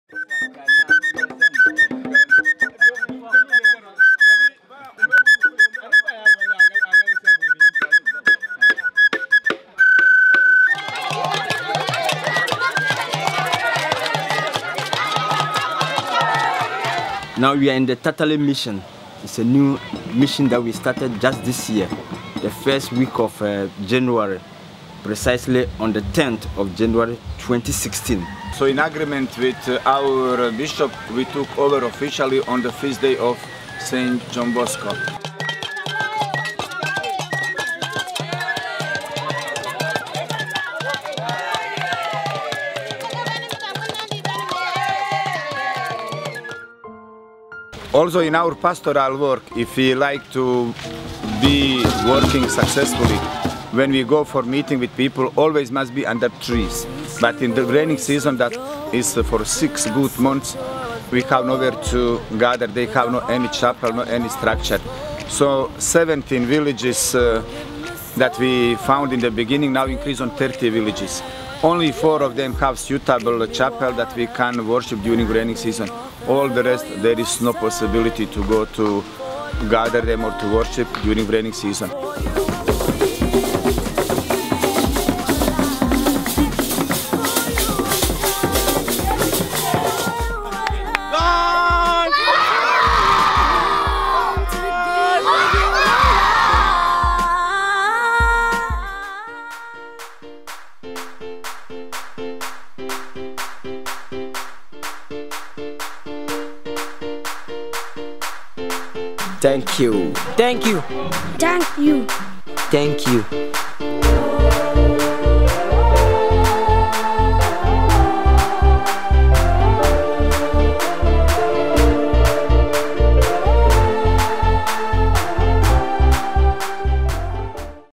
Ghana @25 - Tatale
"When we came to Tatale, in 2016, we had 18 outstations villages including the main parishes but with our presence here, as salesians of Don Bosco, this villages are been increased up to 30 and there are still few more to come (...)
When we arrived we discovered that these villages and their people are very poor so we helped them with schools, scholarships for children and with water because during the dry season is very difficult to get water" - Br. Francis, SDB